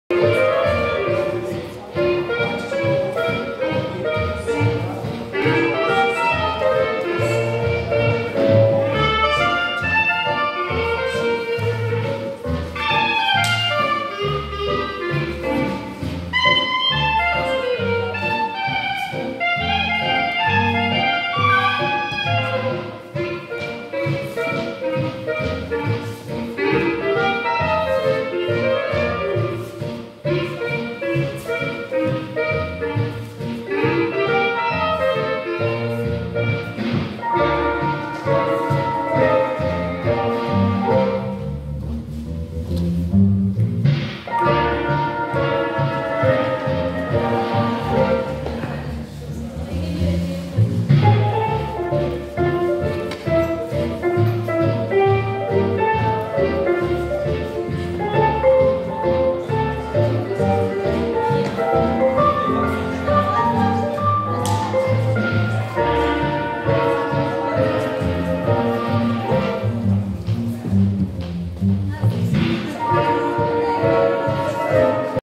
Level 2 Lindy Hop 3.3.20
Swing out from closed
Lindy circle
Rhythm variation 6 count + 2 in open

or

Lindy circle
Lindy circle

Rhythm variation 6 count + 2 in jockey position